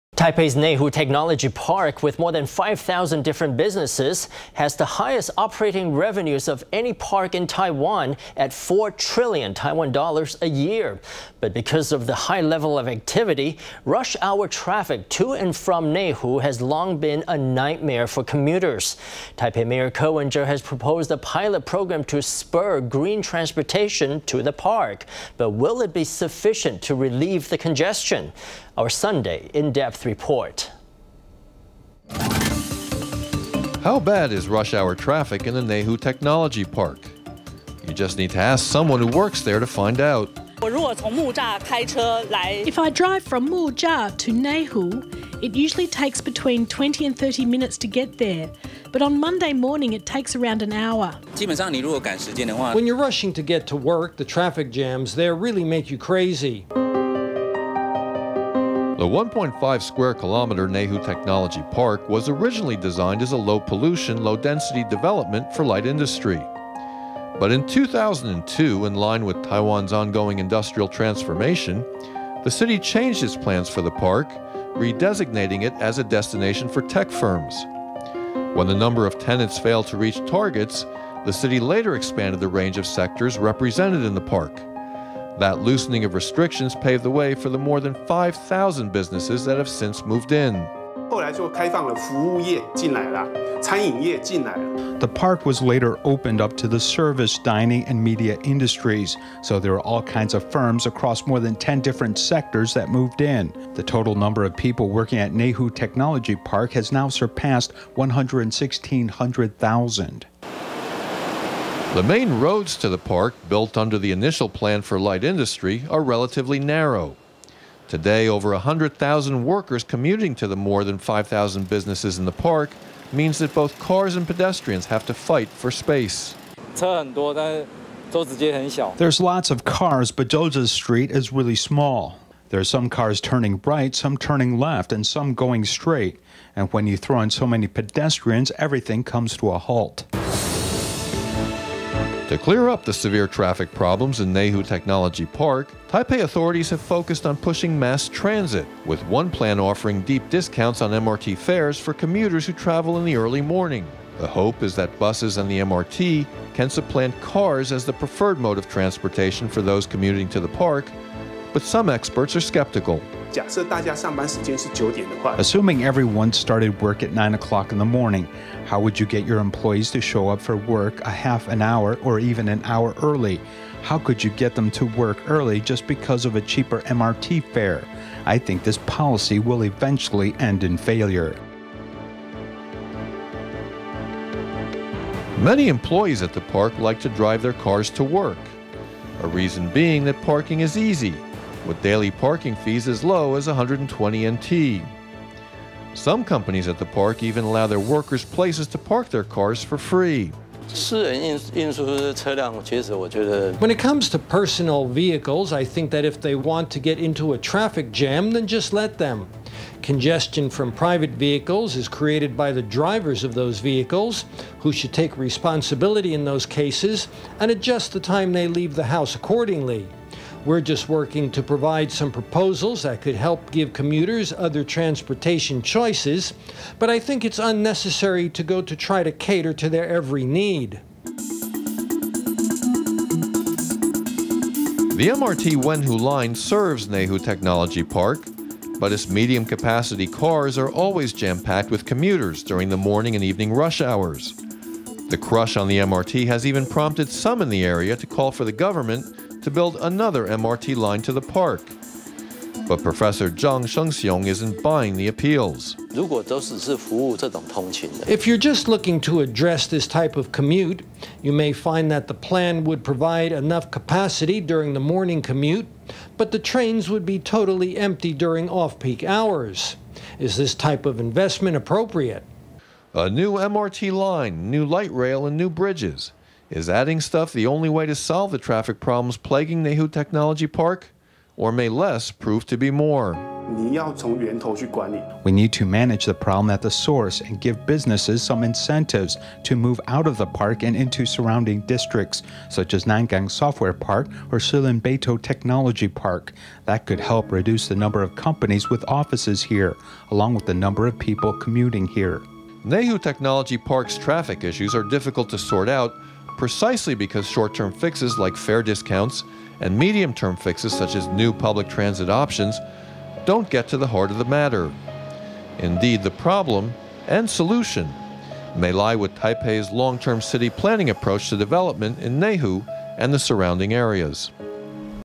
Taipei may need to take long-term view to solve persistent traffic congestion in Neihu
Taipei''s Neihu Technology Park, with more than 5,000 different businesses, has the highest operating revenues of any park in Taiwan at $4 trillion Taiwan dollars a year. But because of the high level of activity, rush hour traffic to and from Neihu has long been a nightmare for commuters. Taipei Mayor Ko Wen-je has proposed a pilot program to spur "green transportation" to the Park, but will it be sufficient to relieve the congestion? Our Sunday in-depth report.How bad is rush-hour traffic in the Neihu Technology Park? You just need to ask someone who works there to find out.Ms. ChangEmployee at Neihu Technology ParkIf I drive from Muzha to Neihu, it usually takes between 20 and 30 minutes to get there, but on Monday morning, it takes around an hour.Mr. TsaiEmployee at Neihu Technology ParkWhen you’re rushing to get to work, the traffic jams there really make you crazy.The 1.5 square-kilometer Neihu Technology Park was originally designed as a low-pollution, low-density development for light industry. But in 2002, in line with Taiwan’s ongoing industrial transformation, the city changed its plans for the park, re-designating it as a destination for tech firms. When the number of tenants failed to reach targets, the city later expanded the range of sectors represented in the park. That loosening of restrictions paved the way for the more than 5,000 businesses that have since moved in.   Ho Shih-changReal Estate AnalystThe Park was later opened up to the service, dining and media industries, so there were all kinds of firms across more than 10 different sectors that moved in. The total number of people working at Neihu Technology Park has now surpassed 116,000. The main roads to the park, built under the initial plan for light industry, are relatively narrow. Today, over 100,000 workers commuting to the more than 5,000 businesses at the park means that both cars and pedestrians have to fight for space.Mr. KuoEmployee at Neihu Technology ParkThere’s lots of cars, but Zhouzi Street is really small. There’s some cars turning right, some turning left, and some going straight, and when you throw in so many pedestrians, everything comes to a halt.To clear up the severe traffic problems in Neihu Technology Park, Taipei authorities have focused on pushing mass transit, with one plan offering deep discounts on MRT fares for commuters who travel in the early morning. The hope is that buses and the MRT can supplant cars as the preferred mode of transportation for those commuting to the park, but some experts were skeptical.Ho Shih-changReal Estate AnalystAssuming everyone started work at nine o’clock in the morning, how would you get employees to show up for work a half an hour, or even an hour, early? How could you get them to go to work early just because of a cheaper MRT fare? I think this policy will eventually end in failure.Many employees at the park like to drive their cars to work, one reason being that parking is easy, with daily parking fees as low as NT$120. Some companies at the park even allow their workers places to park their cars for free.Chang Sheng-hsiungTKU Dept. of Transportation ManagementWhen it comes to personal vehicles, I think that if they want to get into a traffic jam then just let them. Congestion from private vehicles is created by the drivers of those vehicles, who should take responsibility in those cases and adjust the time they leave the house accordingly. We’re just working to provide some proposals that could help give commuters other transportation choices, but I think it’s unnecessary to go try to cater to their every need.The MRT Wenhu line connects to Neihu Technology Park, but its medium-capacity cars are always jam-packed with commuters during the morning and evening rush hours. The crush on the MRT has even prompted some in the area to call for the government to build another MRT line to the Park, but Professor Chang Sheng-hsiung, a professor in Tamkang University’s Department of Transportation Management, isn’t buying the appeals.Chang Sheng-hsiungTKU Dept. of Transportation ManagementIf you’re just looking to address this type of commute, you may find that the plan would provide enough capacity during the morning commute, but the trains would be totally empty during off-peak hours. Is this type of investment appropriate?A new MRT line, new light rail, and new bridges – is adding stuff the only way to solve the traffic problems plaguing Neihu Technology Park? Or may less prove to be more?Ho Shih-changReal Estate AnalystWe need to manage the problem at the source, and give businesses some incentives to move out of the park and into surrounding districts, such as Nangang Software Park or the Shilin-Beitou Technology Park. That could help reduce the number of companies with offices here, along with the number of people commuting here.Neihu Technology Park’s traffic issues are diff...